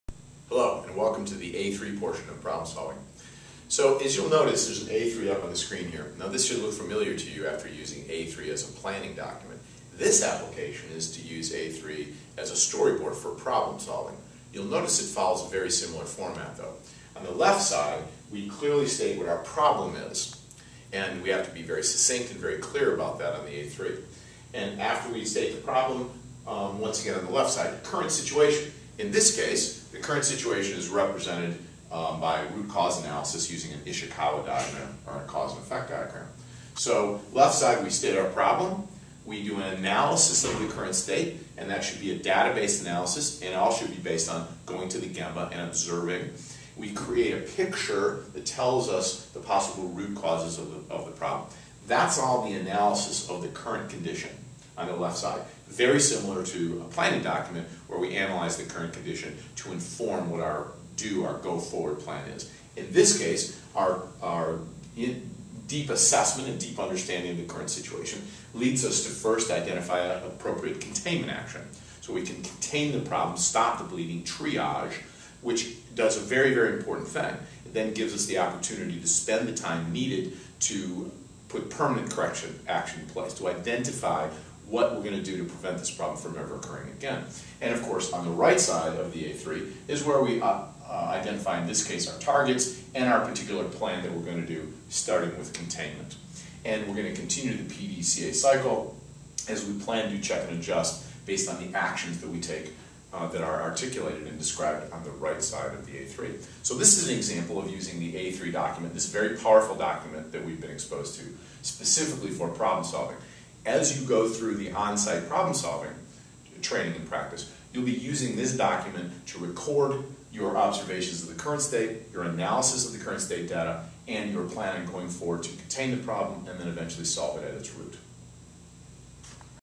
A3 Problem Solving Detailed
Using the A3 format for Problem Solving planning and documentation.